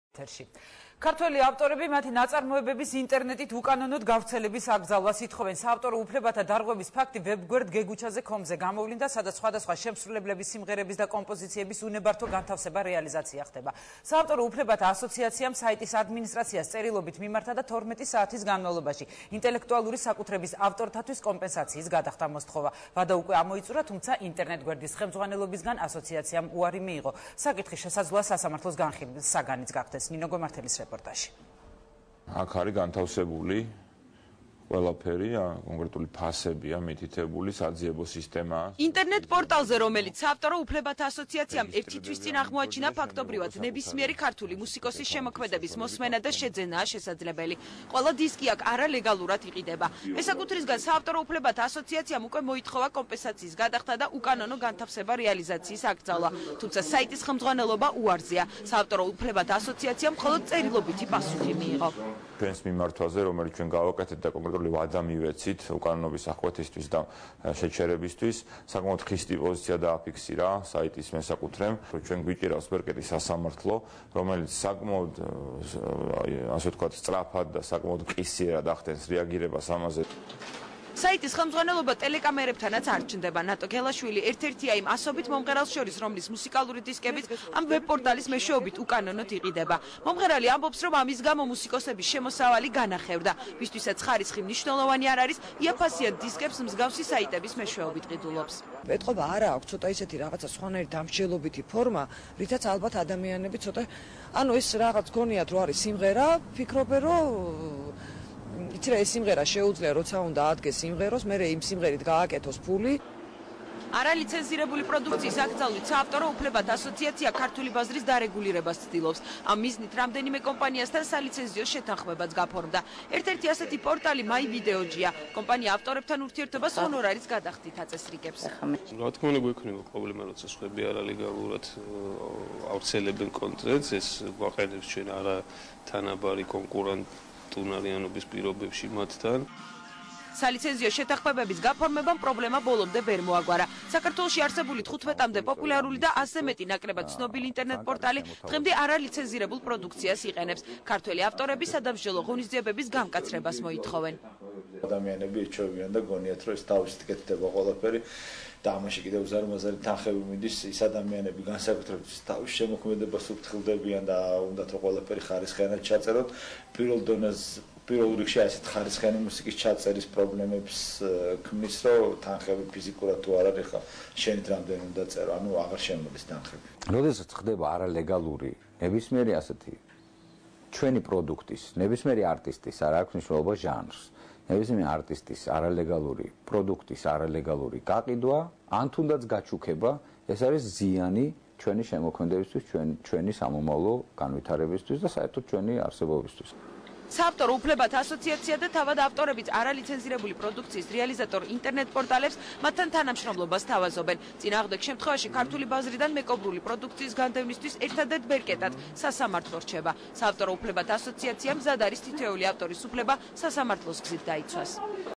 Authors Demand the Elimination of Internet Allocation of Their Works
GCA applied to the website owner with the request to stop the illegal allocation/realization of works and required certain compensation for the authors. Regarding the issue, on the 28th of March, Giga Kobaladze - the Chairman of GCA held the special press conference.

Prohibition of illegal content allocation at the internet is one more step forward on the way of internet legalization process in Georgia. The campaign aims the replacement of illegal production with the legal one, which eventually will lead to the withdrawal of piracy production from the Georgian market. All the legal tools will be effective for the protection of authors' copyright. The court sue may be an optional solution for the issue.